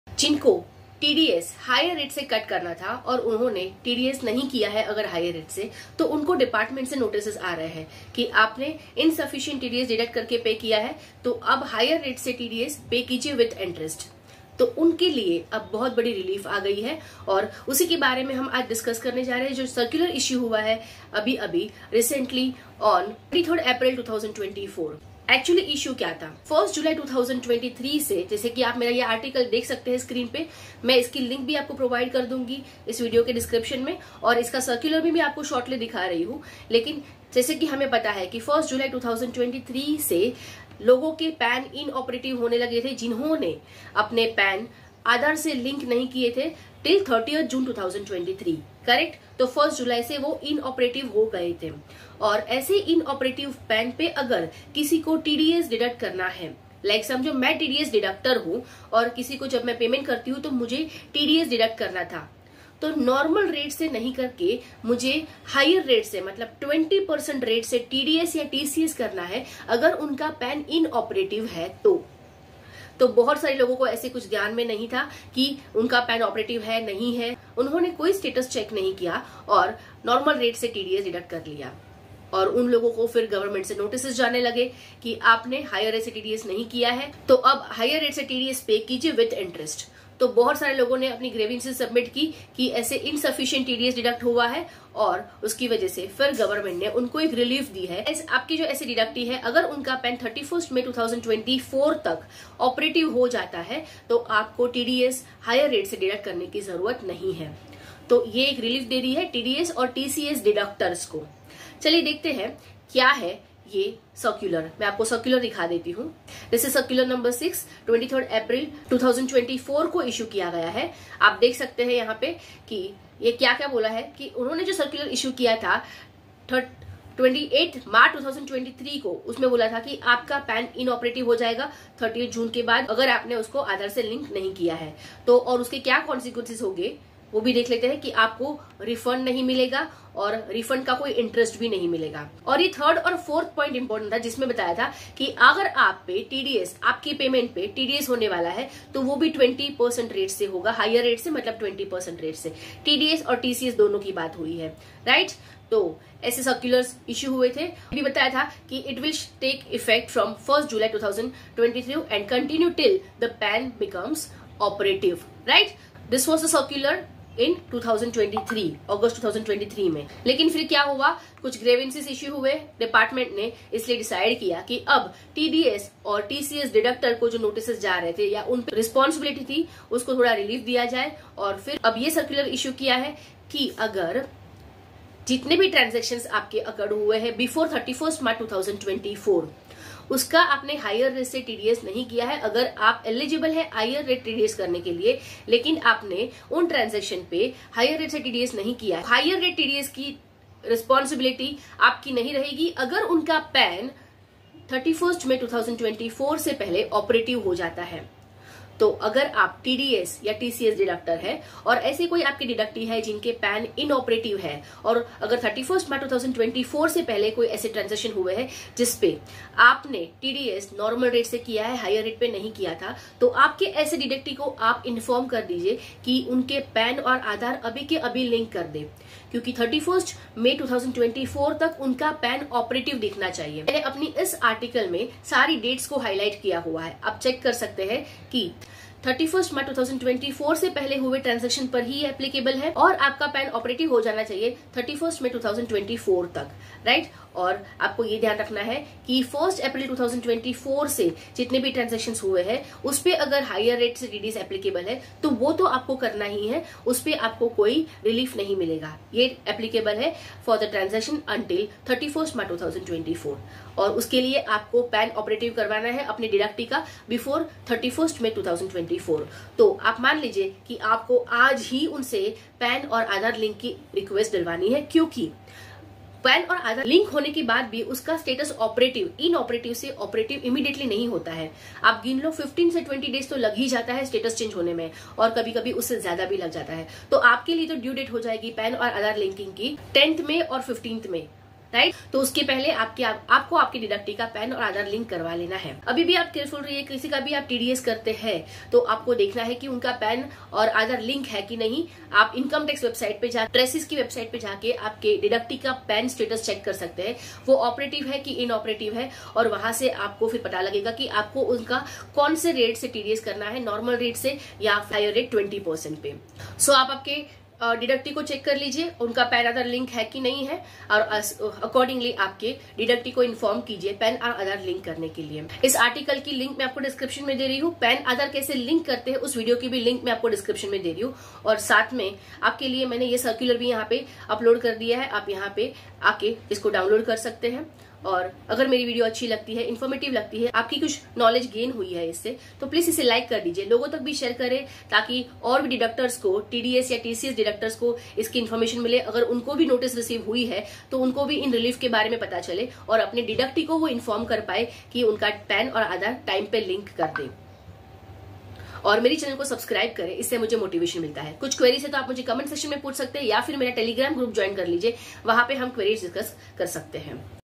Date extended Link PAN Aadhaar | TDS return default notice relief | PAN inoperative
CBDT Circular No. 6: Relief for TDS/TCS Deductors on PAN-Aadhaar Linkage

The Central Board of Direct Taxes (CBDT) has taken a significant step towards addressing the grievances of deductors and collectors who have encountered challenges due to the inoperability of PAN linked with Aadhaar. Circular No. 6, issued on April 23, 2024, aims to provide relief to those who have collected Tax Deducted at Source (TDS) or Tax Collected at Source (TCS) at the normal rate but were required to deduct or collect at higher rate due to PAN inoperability.